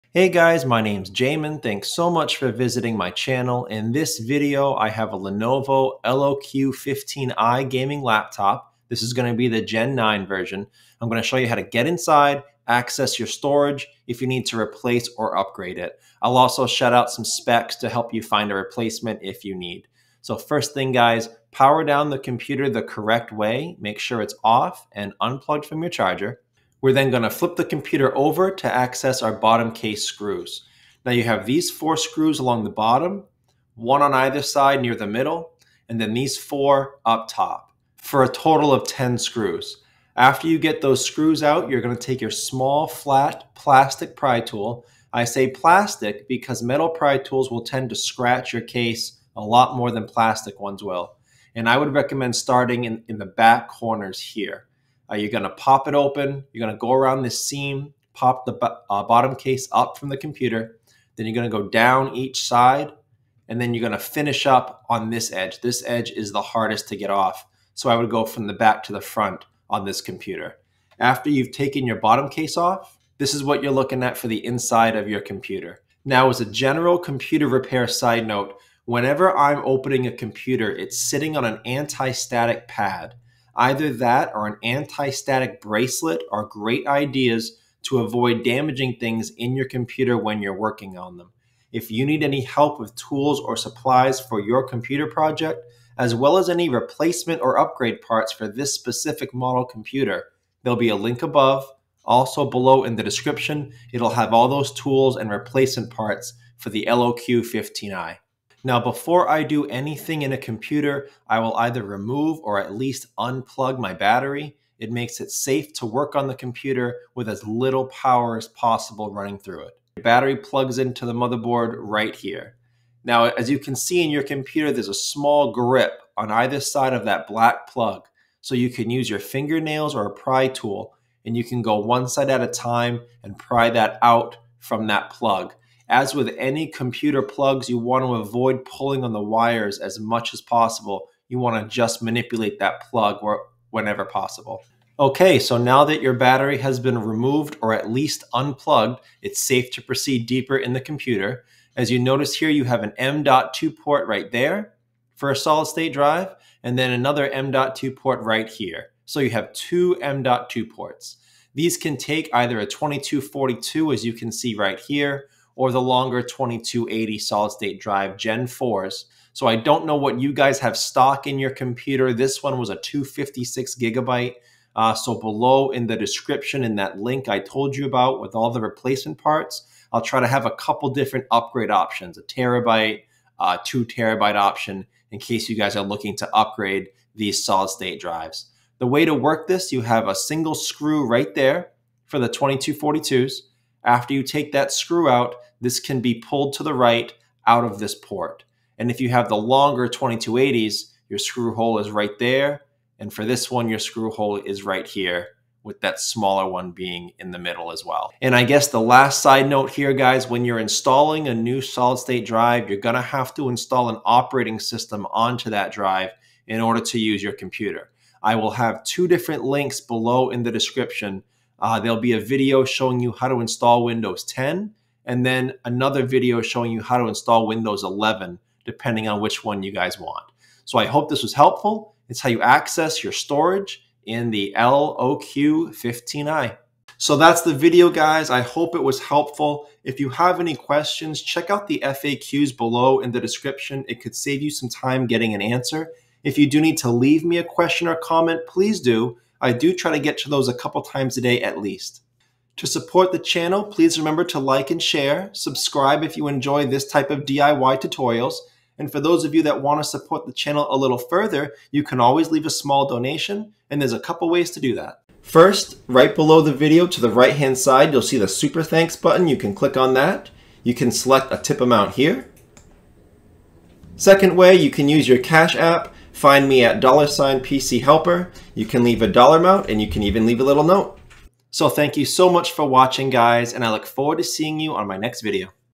How To Replace Upgrade SSD - Lenovo LOQ 15i (Gen9) Gaming Laptop Computer
In this video, Ill show you how to safely access the Storage / SSD in your Lenovo LOQ 15i series laptop computer. Note: If you desire more speed / performance, consider upgrading the RAM as well.
Lenovo LOQ 15i Tools & Replacement Parts

FAQ's:
SSD Info: 2x M.2 2242 or 2280 Gen4 SSD's